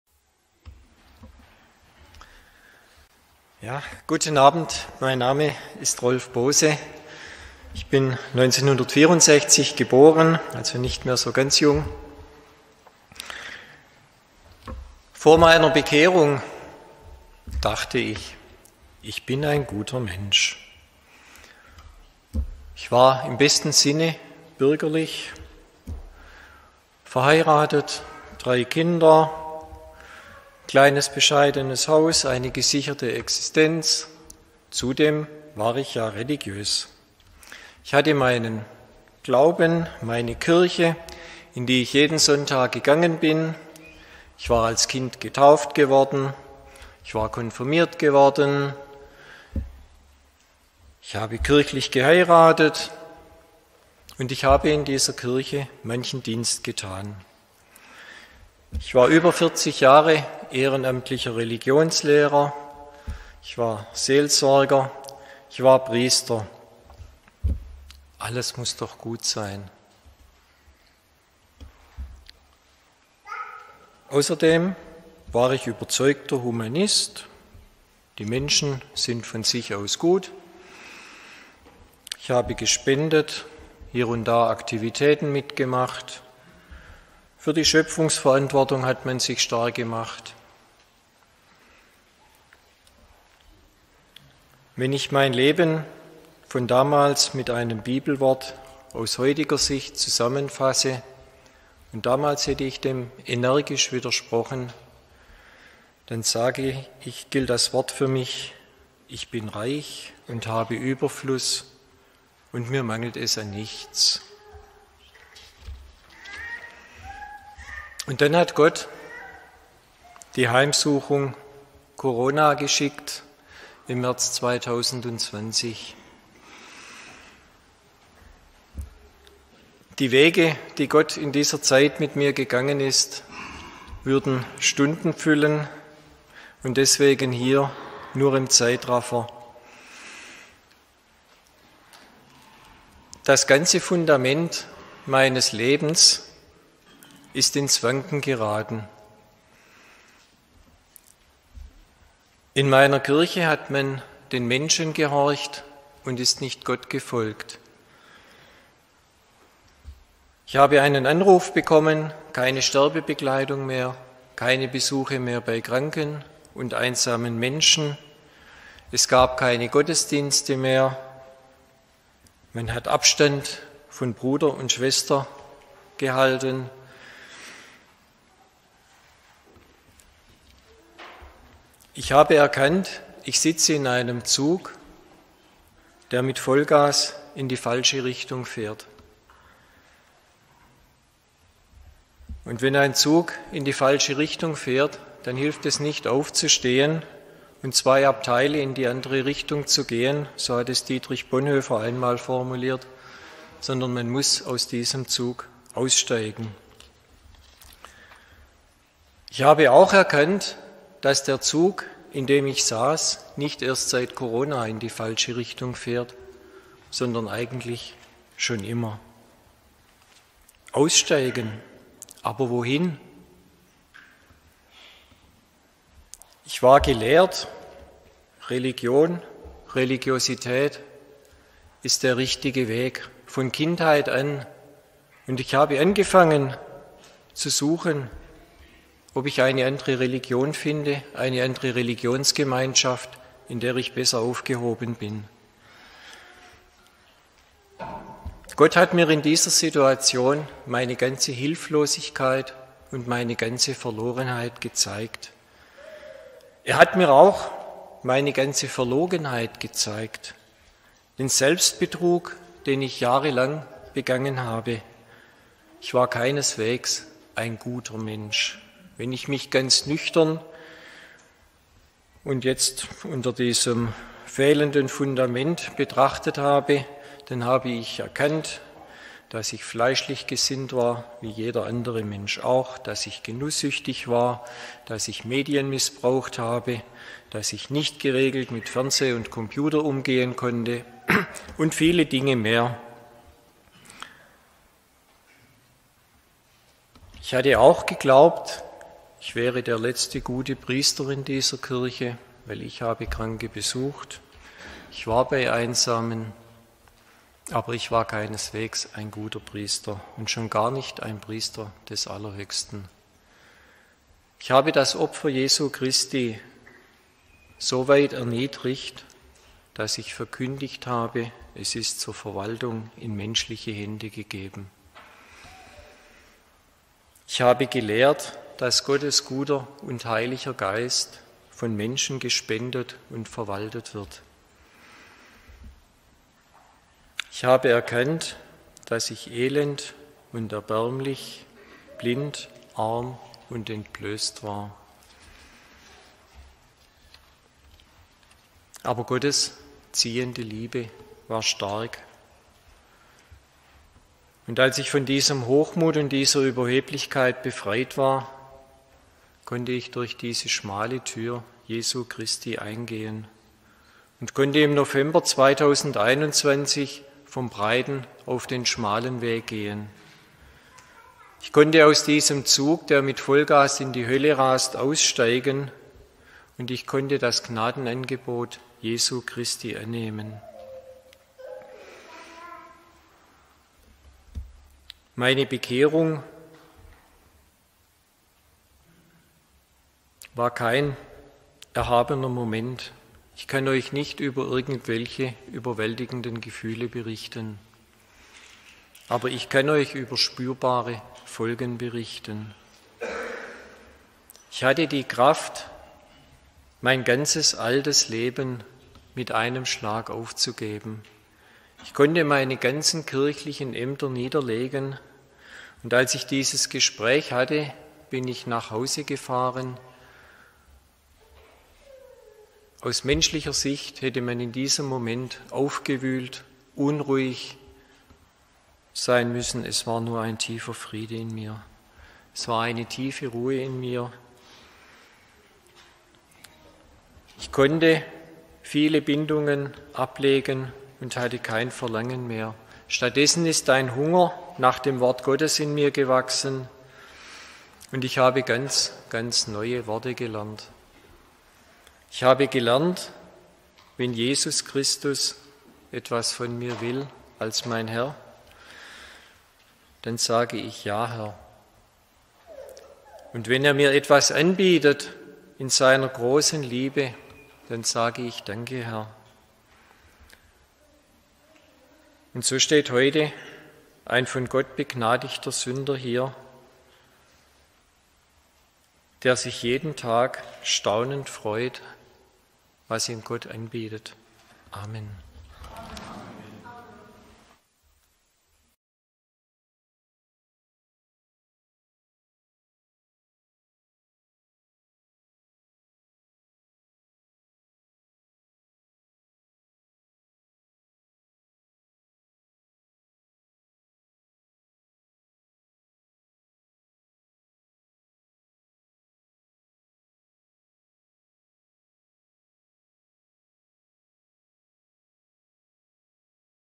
Von der NEUAPOSTOLISCHEN KIRCHE zum BIBLISCHEN GLAUBEN. Ex-Priester erzählt
Er war viele Jahre Priester und Religionslehrer der Neuapostolischen Kirche. Jetzt bezeichnet er sich als bibeltreuer Christ. Hören Sie das Lebenszeugnis von Bruder Rolf Bose während einer Evangelisation in der Bibelgemeinde Pforzheim. Hören Sie zur Vertiefung die Vortragsreihe von Dr. Lothar Gassmann: NEUAPOSTOLISCHE KIRCHE.  ECHTE ODER FALSCHE APOSTEL?